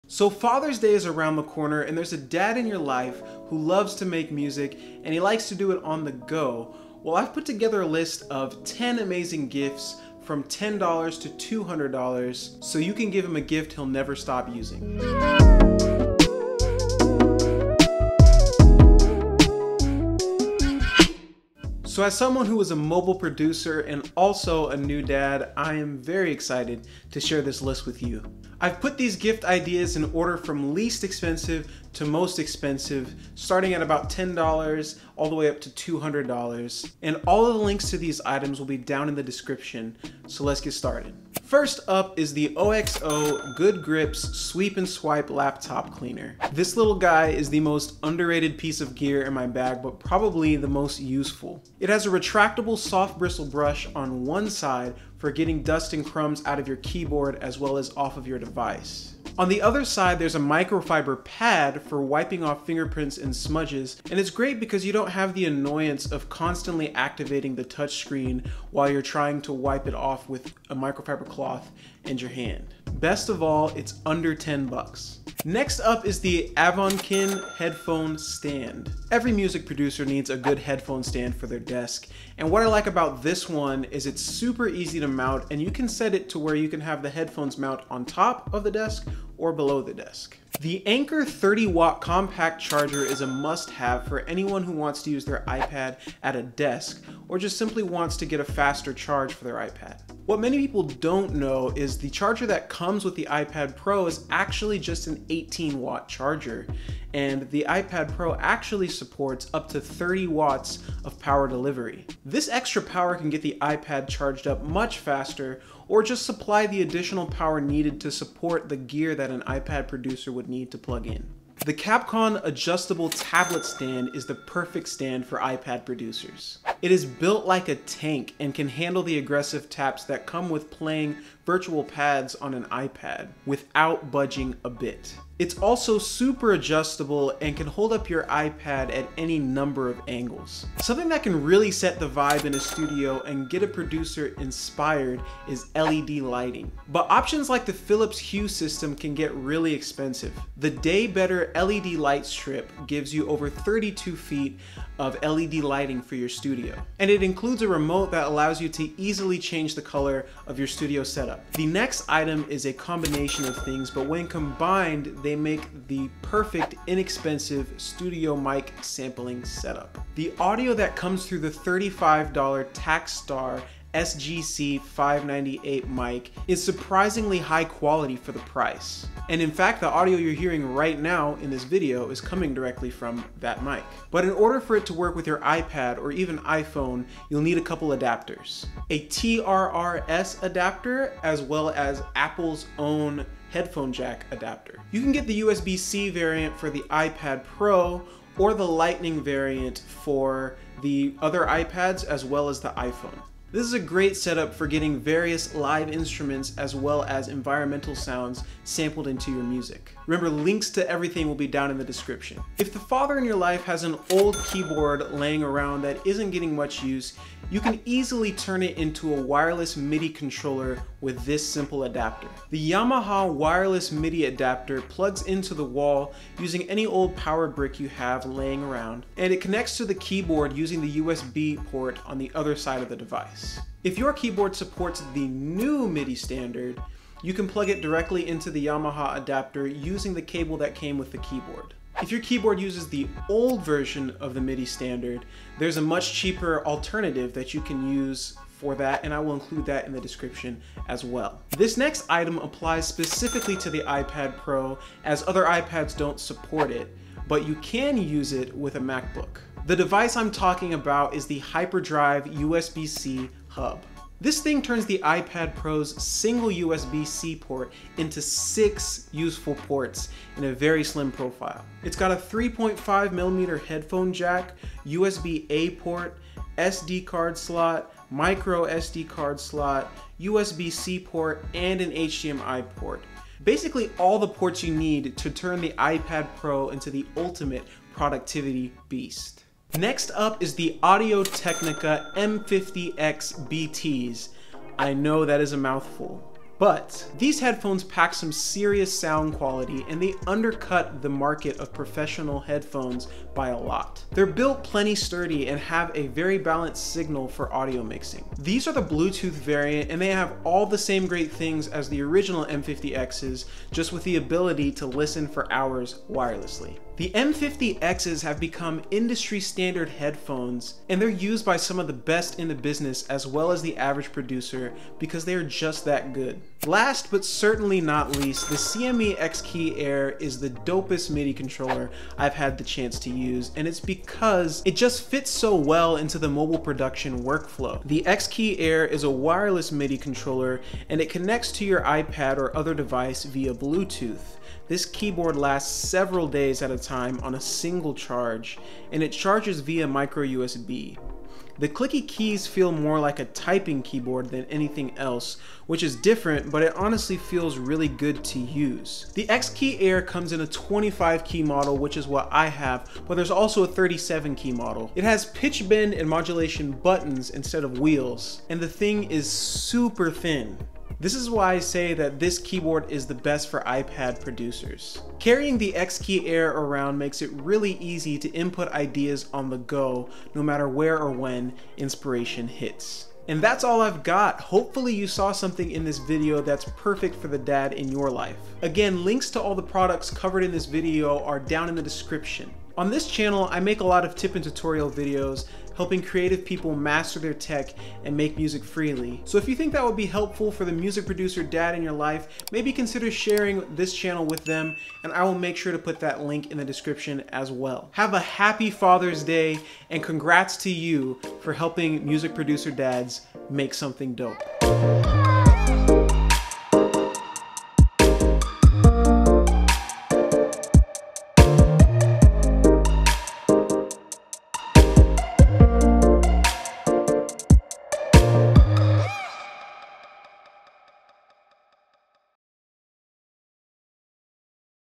10 AMAZING FATHER’S DAY GIFTS ($10-$200) for Mobile Music Producer Dads
10 AMAZING FATHER’S DAY GIFTS ($10-$200) for Mobile Music Producer Dads

PRODUCT LINKS: ✅
➡️ OXO Sweep & Swipe
➡️ Headphone Stand
➡️ Anker 30w Compact Charger
➡️ Kabcon iPad Stand
➡️ Daybetter LED Light Strip
➡️ TAKSTAR SGC-598
➕ TRRS Adapter
➕Apple USB-C Headphone adapter
      OR
➕Lightning Headphone adapter
➡️ Yamaha Wireless Midi Adapter
OR HXW for old version of MIDI
➡️ Hyperdrive USB-C Hub
➡️ Audio-Technica ATH-M50xBT
➡️ CME XKEY Air